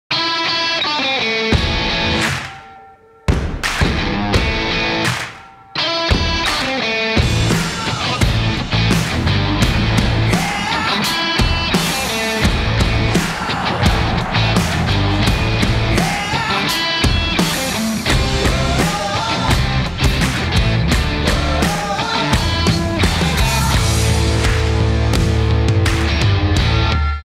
L'Action du match
L' ActionDuMatch de la rencontre ASMR92 avec le coup d’œil et l'exécution technique parfaite de Camille Lopez qui adresse une merveille de coup de pied dans le fond du terrain où Rémy Grosso n'a plus qu'à récupérer pour aplatir. Retour en images grâce à notre partenaire PAPREC.